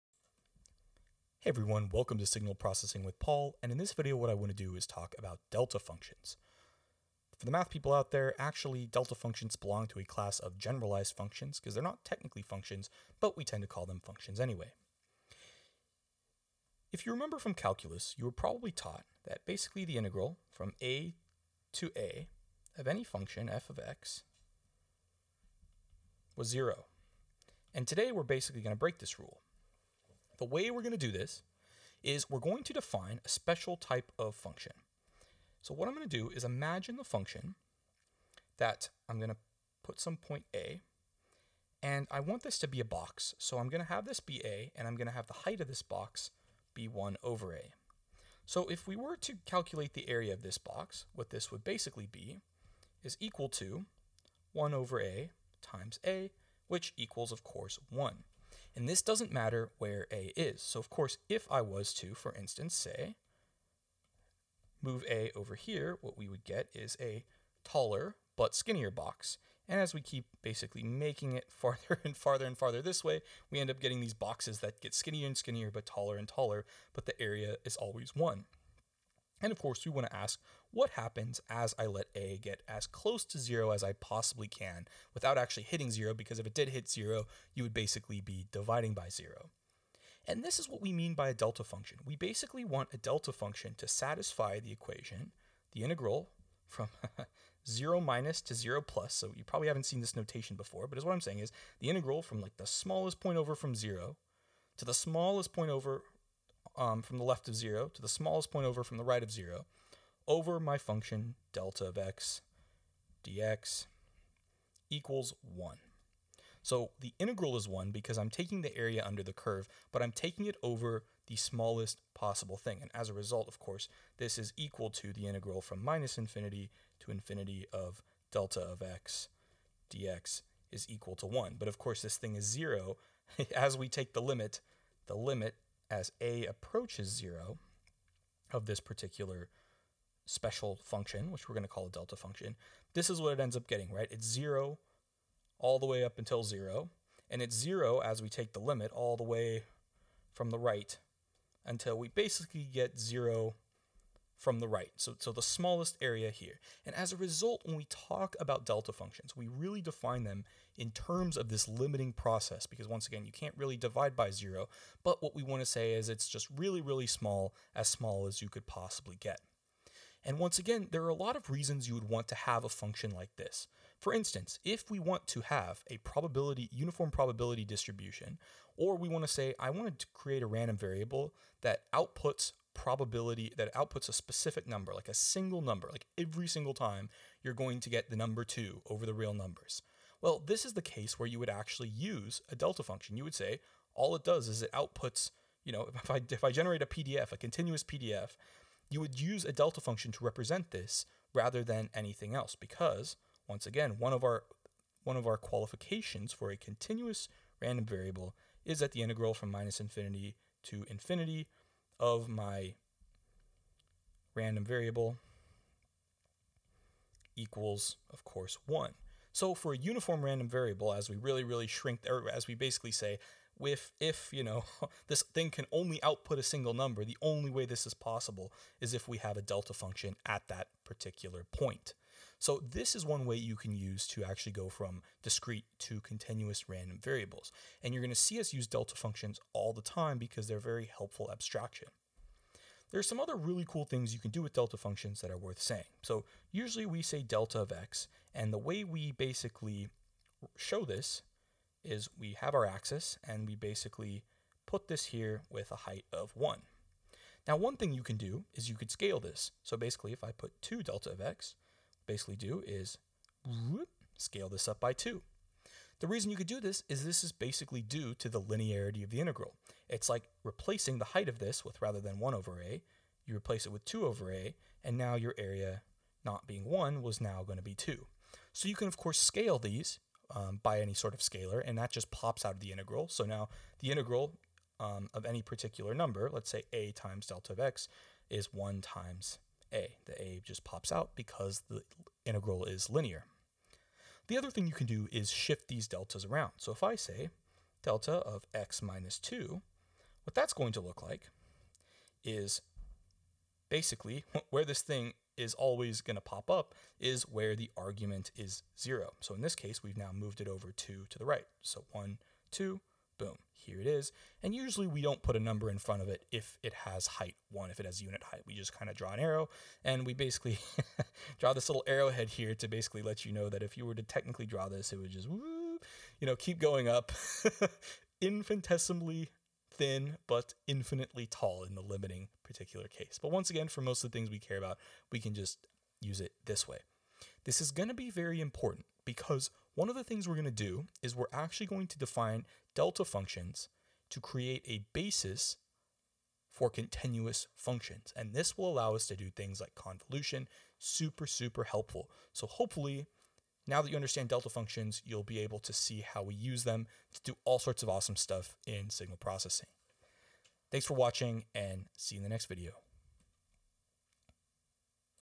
Delta Functions - What they are (and why you should care)
In this video, I'll explain what delta functions are and why you should care about them.